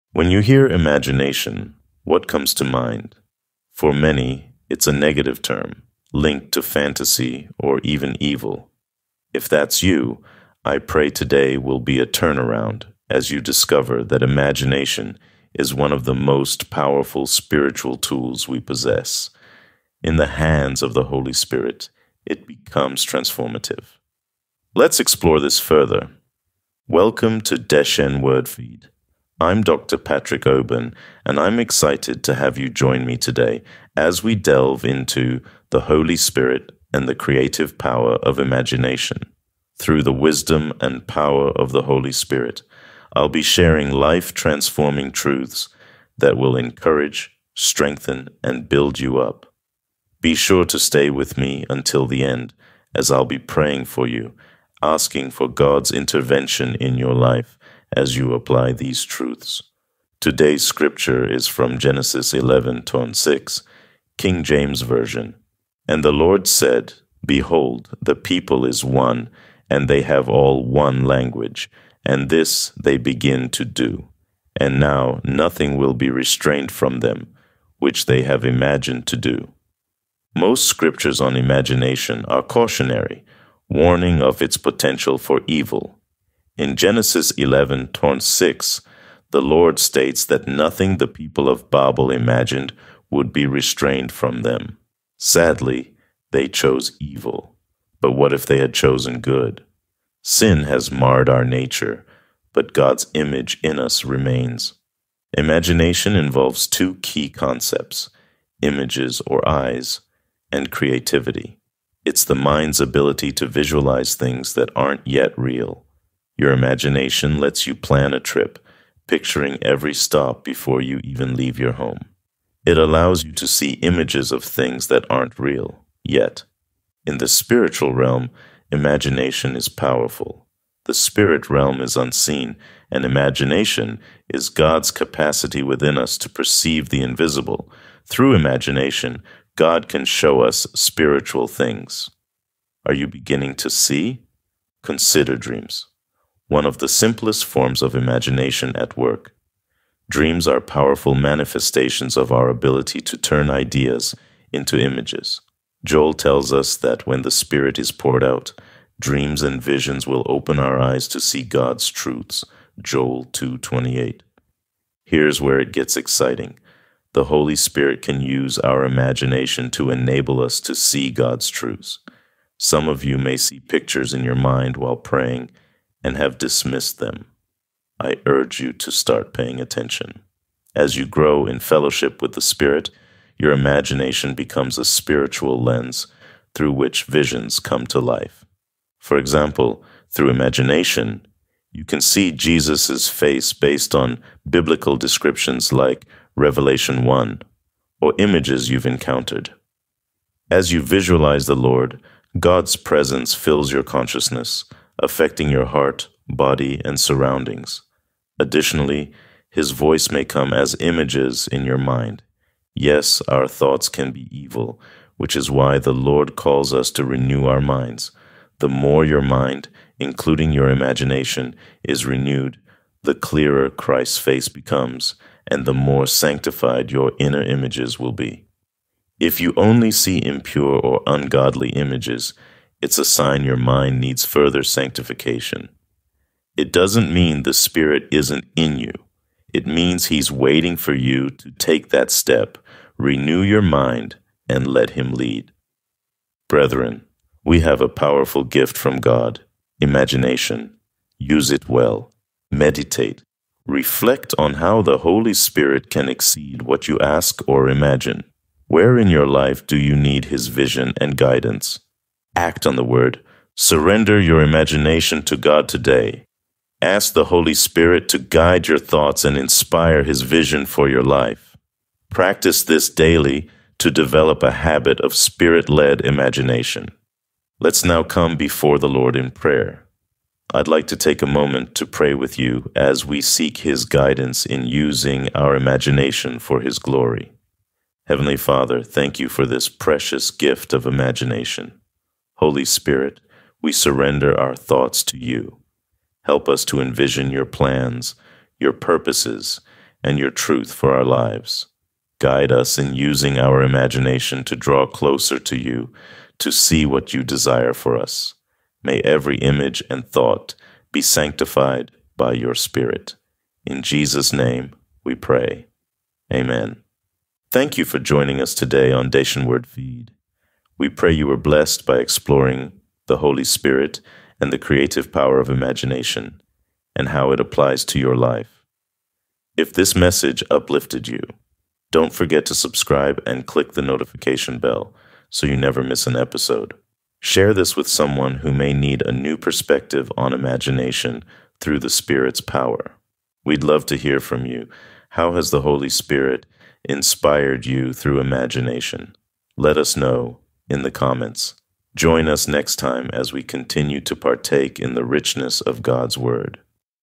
The Holy Spirit and the Creative Power of Imagination | Deshen Daily | Nov 4, 2024
When you hear "imagination," what comes to mind? For many, it’s a negative term, linked to fantasy or even evil. If that’s you, I pray today will be a turnaround as you discover that imagination is one of the most powerful spiritual tools we possess.